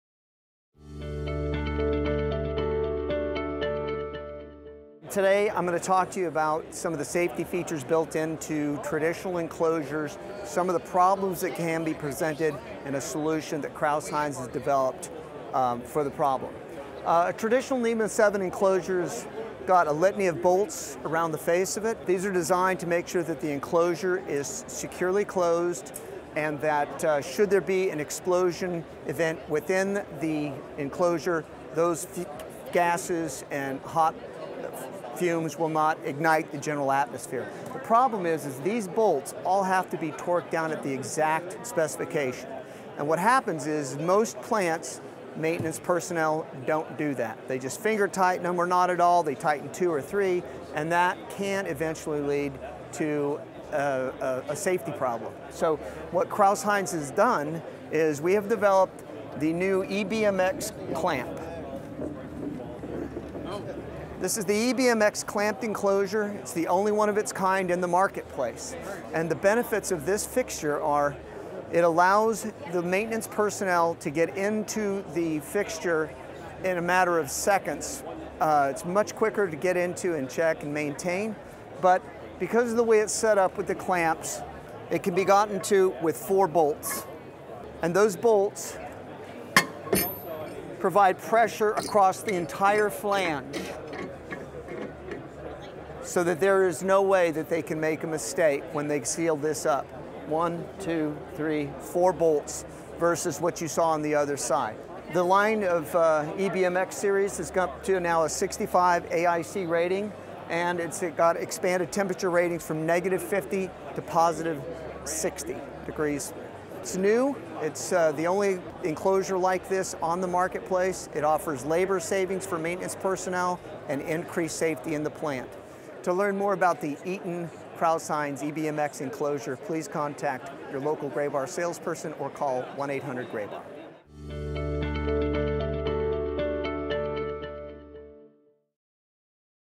NEMA 7 Classified Clamped EBMX Enclosures from Eaton Crouse-Hinds
Some of the safety features built in to traditional NEMA 7 enclosures can be cumbersome, such as the bolts around the face of the enclosure.

They are designed to ensure that the enclosure is securely closed if there should be an explosion event within the enclosure;, that those gases and hot fumes will not ignite the general atmosphere. The problem is, the bolts all have be torqued down at the exact specification, but that doesn't always happen and that could lead to a safety problem.

Crouse-Hinds has developed a solution: the new EBMX Clamp Enclosure.  It's the only one of it's kind in the marketplace.

Two of the benefits of this fixture are that it allows maintenance personnel to get into the fixture in a matter of seconds, and it's much quicker get into and check and maintain.

Because of the way it's set up with the clamps, the EBMX uses four bolts that provide pressure across the entire flange. That makes it virtually impossible to make a mistake when you are sealing this up.

The line of EBMX series is up to a 65 AIC rating and it's got expanded temperature ratings from -50 to +60 degrees.

It offers labor savings for maintenance personnel and increased safety in the plant.